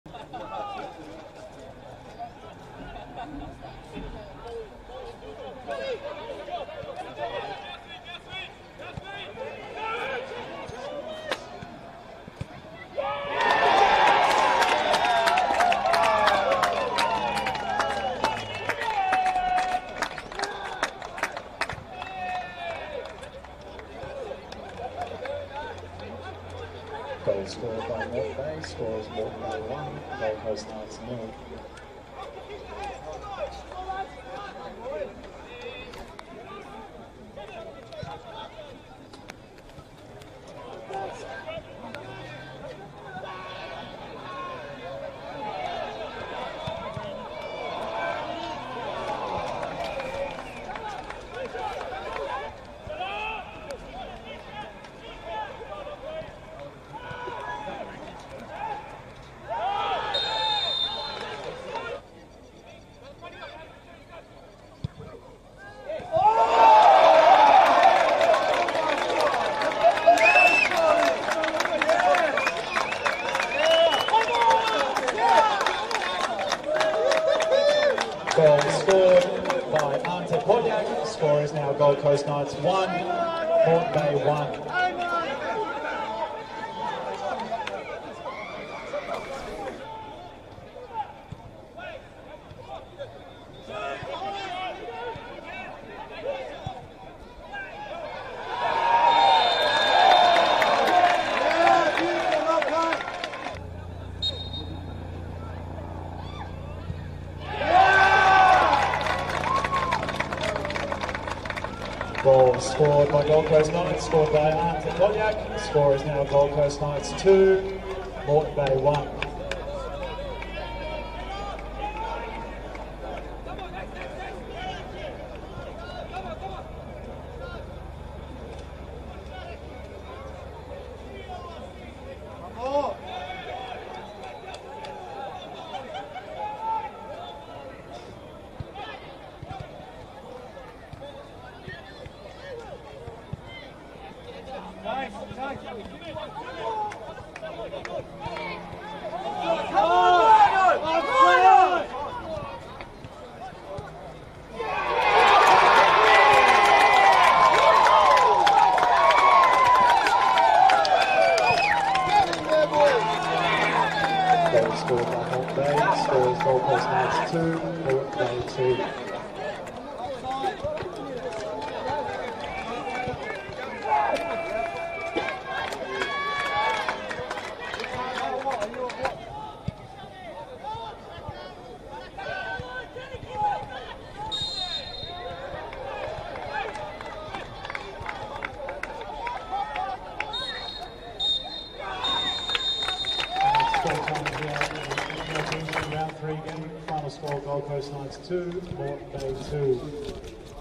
NPL Men R3 - Gold Coast Knights vs. Moreton Bay United Highlights
5:15 PM - Saturday, 11 March

NPLQLD Men